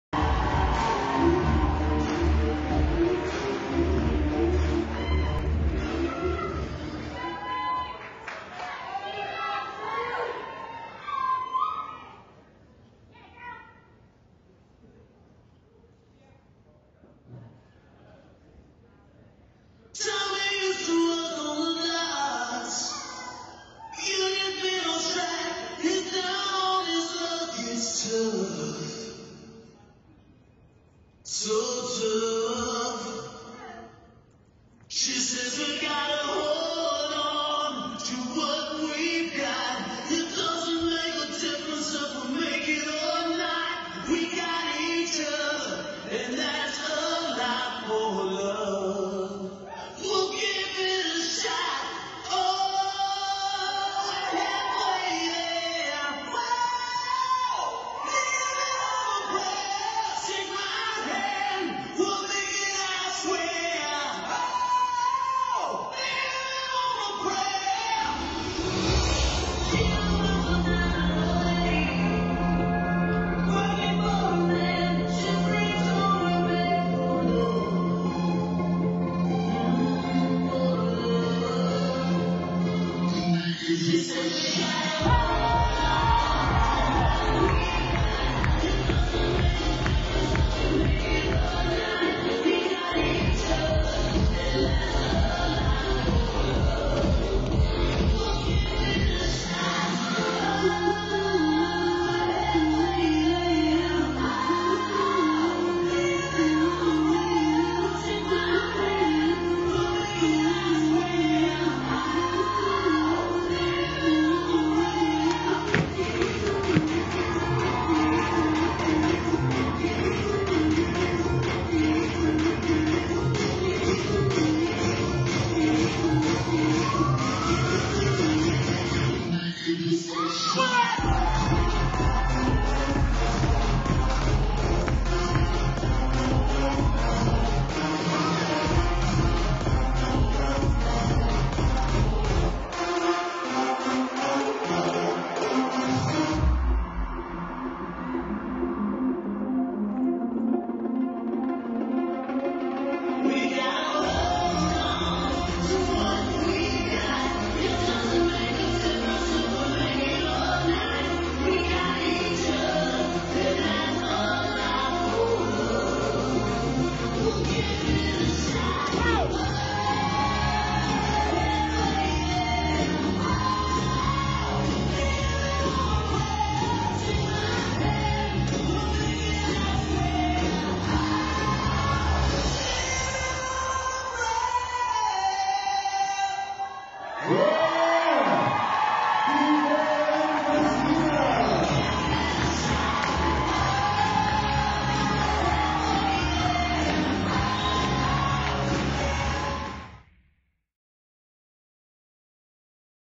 Liberty Swing 2016 Classic PJ Turner & Tashina Beckmann
Official event videos recorded at the 2016 Liberty Swing Dance Championships in New Brunswick, NJ. This annual event features West Coast Swing competitions, workshops and 4 nights of social dancing.

In 2016 production resources and videography services provided by Dance Jam Productions.